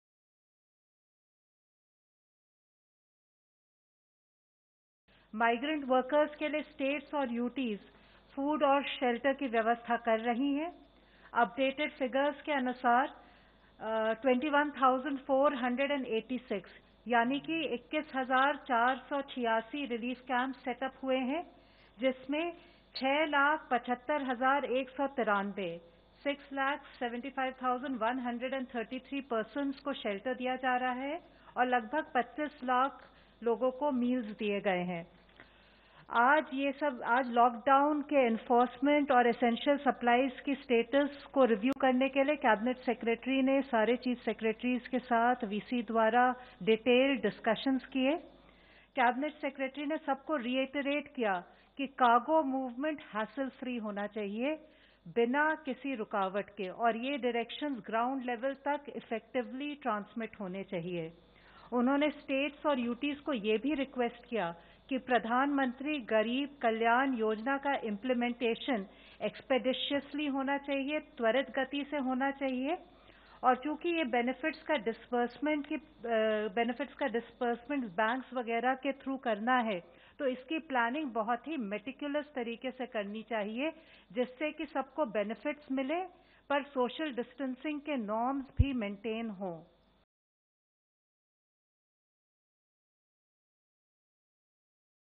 Home Ministry assures to set up 21,486 relief camps in states and UTs
New Delhi, Apr 01(ANI): Home Ministry, Joint Secretary, Punya Salila Srivastava said that the states are arranging food and shelter for the migrant workers and so far 21,486 relief camps have been set up, where around 6,75,133 people have been given shelter. She further said that the Cabinet Secretary has instructed hustle free cargo movement and implementation of all directions on ground level. Along with this, the Joint Secretary added that the states and UTs have also been instructed for expeditious usage of Pradhan Mantri Garib Kalyan Yojana.